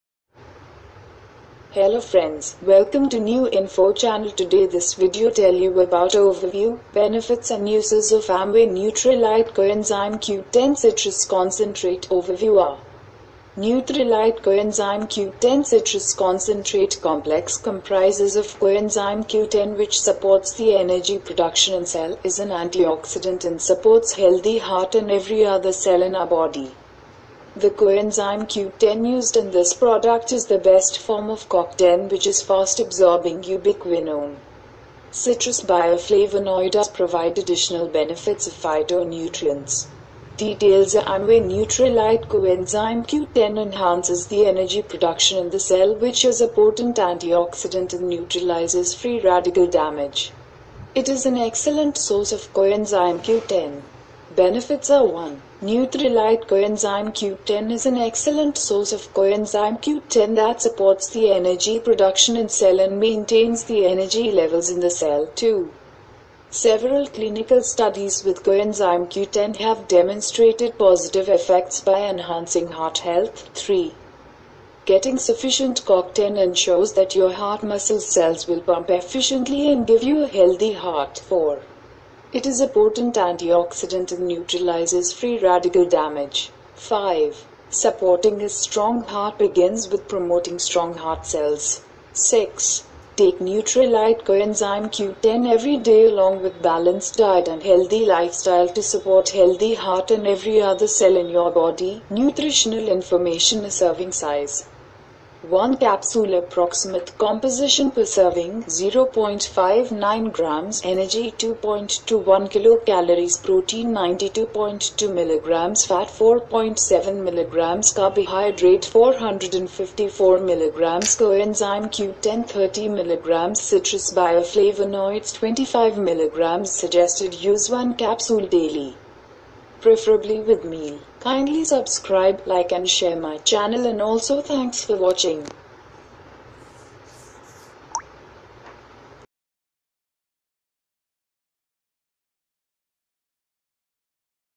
Amway Nutrilite Coenzyme Q10 Citrus Concentrate || Benefits || Use
Dear Friends,
This video tell you about benefits and use of NUTRILITE Coenzyme Q10 citrus concentrate . as it supports the energy production in cell, is an antioxidant and supports healthy heart and
every other cell in our body.
New info channel provide different info in various categories